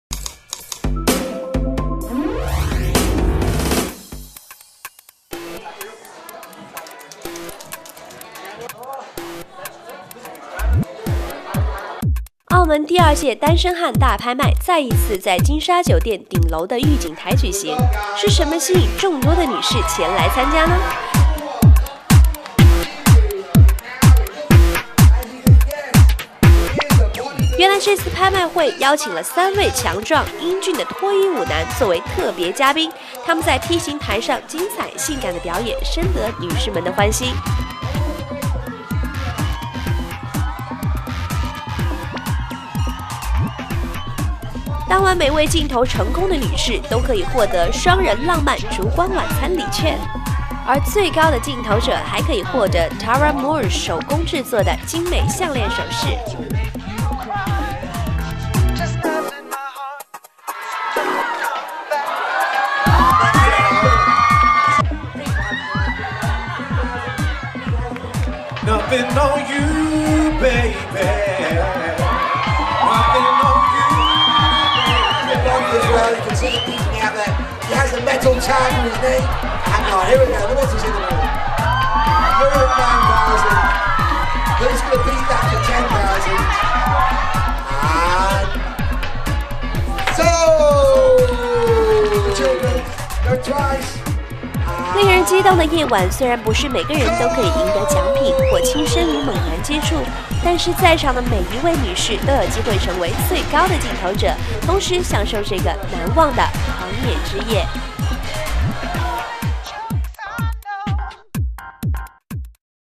2010 年单身汉大拍卖
澳门的女士们最近会被特别款待...第二届单身汉拍卖会将会以慈善形式让全城女子竞投。 这次拍卖会邀请了3位强壮，英俊的脱衣舞男作为特别嘉宾， 他们在T型台上精彩，性感的表演深得女士们的欢心。 令人激动的夜晚，虽然不是每个人都可以赢得奖品或亲身与猛男接触， 但是在场的每一位女士都有机会成为最高竞投者， 同时享受这个难忘的狂野之夜！